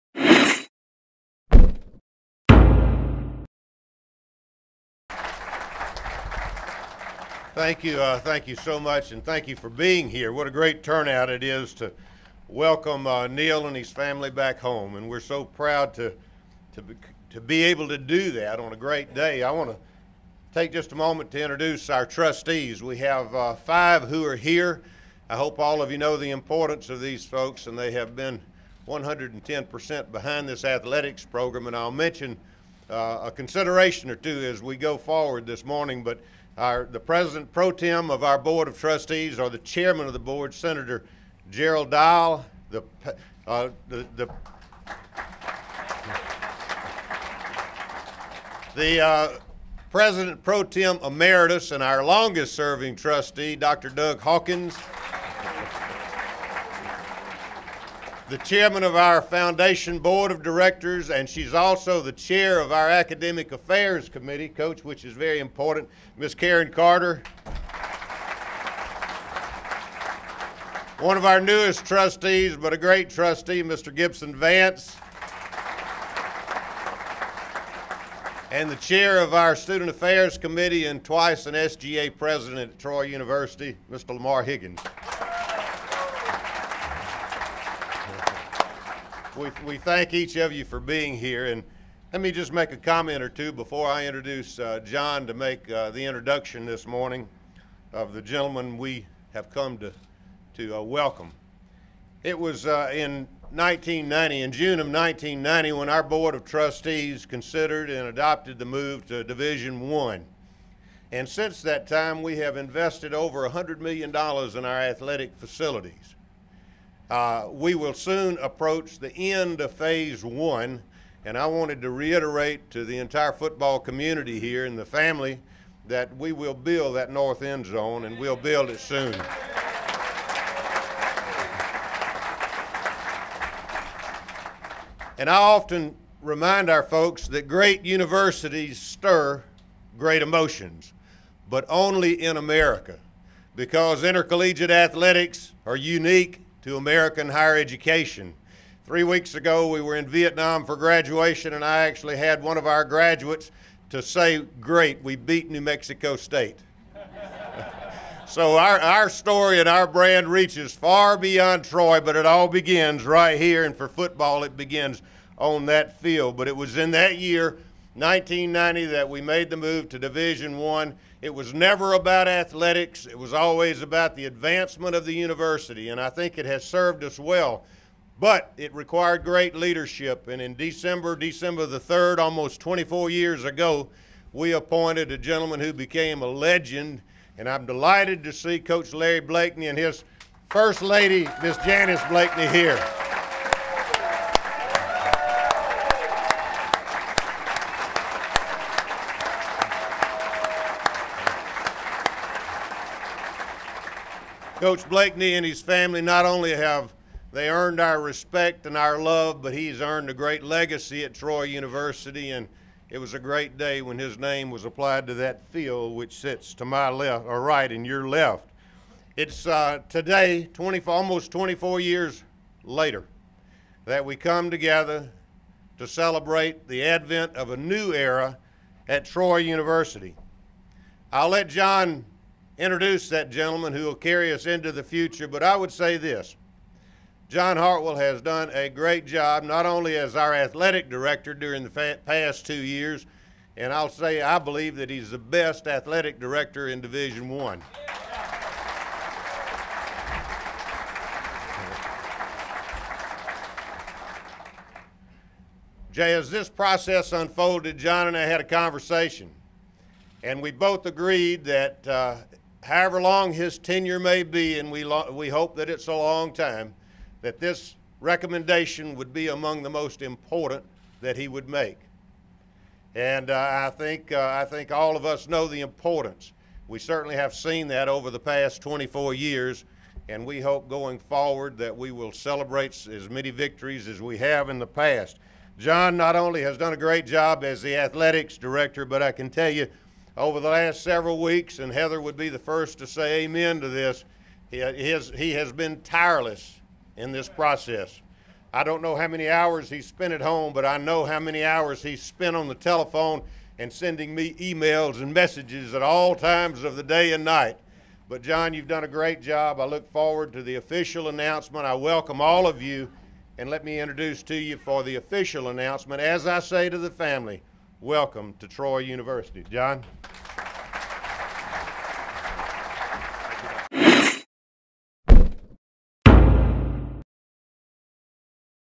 Troy Chancellor Dr. Jack Hawkins, Jr. (Neal Brown press conference)
Troy Chancellor Dr. Jack Hawkins, Jr. addresses the media as Neal Brown is introduced as the school's new football head coach.